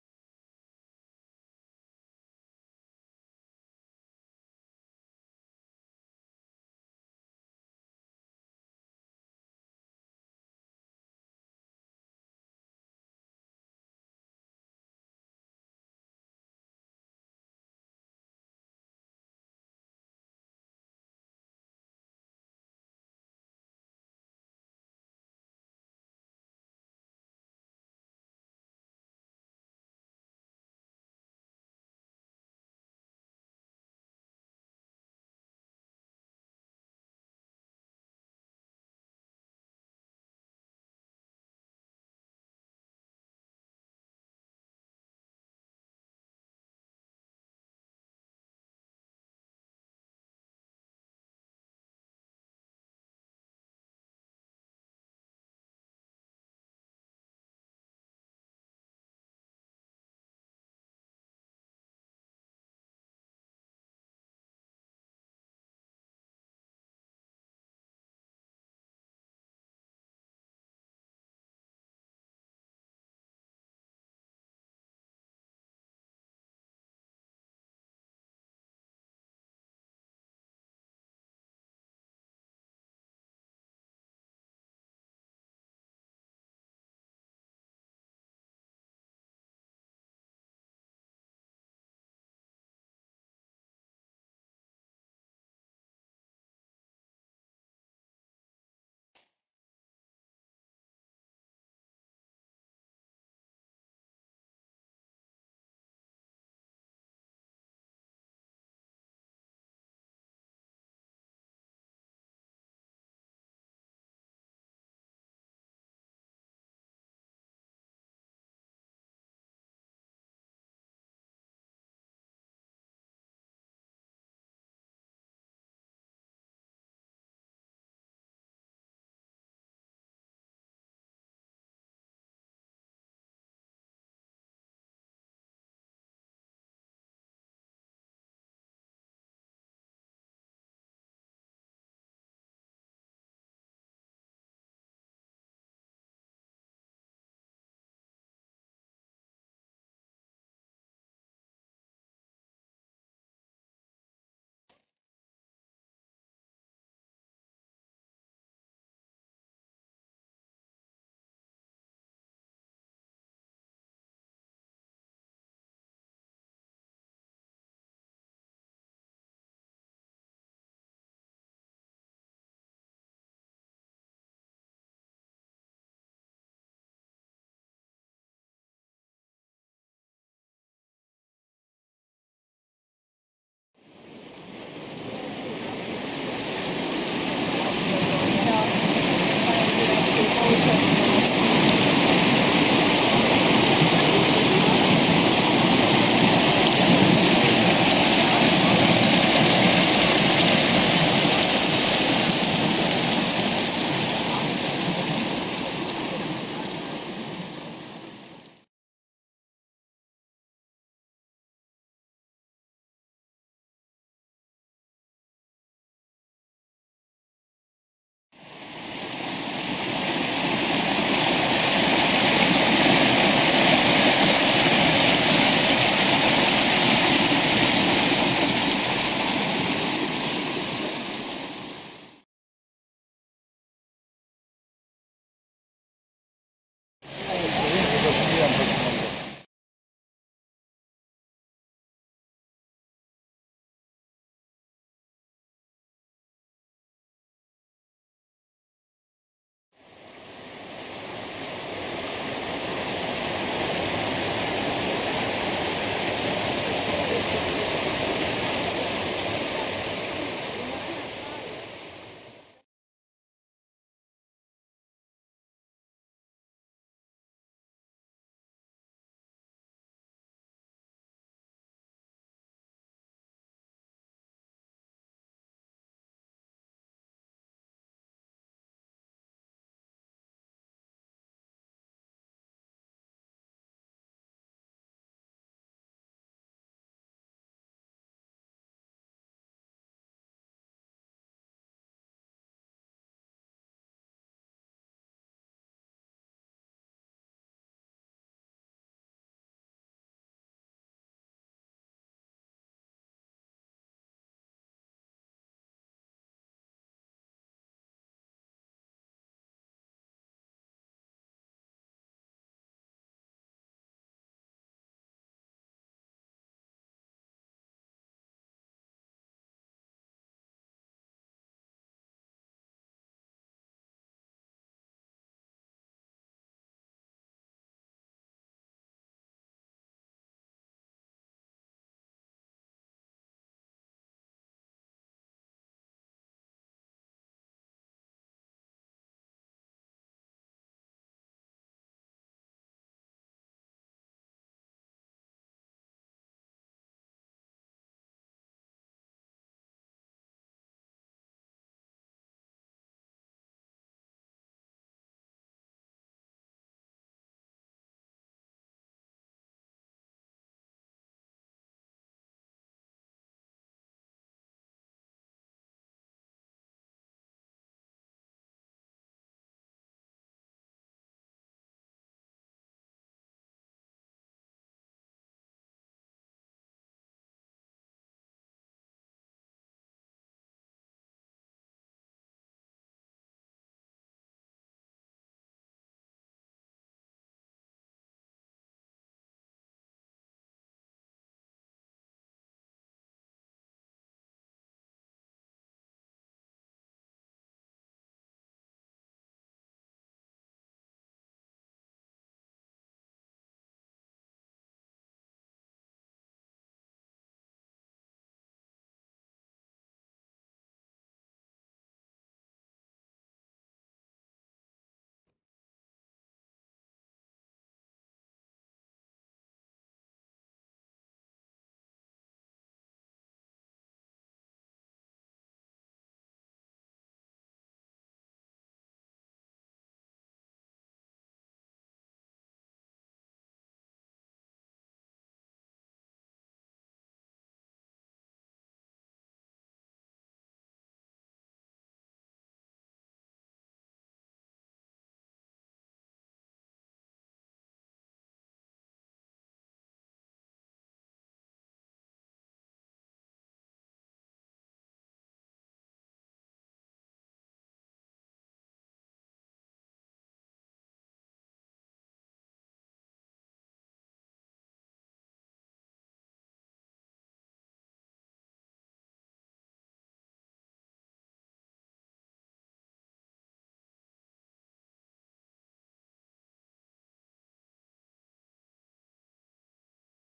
Bohinj (Voje) - 27. 7. 2022
Spoznavanje vrednot pri Društvu Univerza za tretje življenjsko obdobje Sevnica, Četrtkova skupina Planinskega društva Lisca Sevnica, Sredini fotografi. Podrobnosti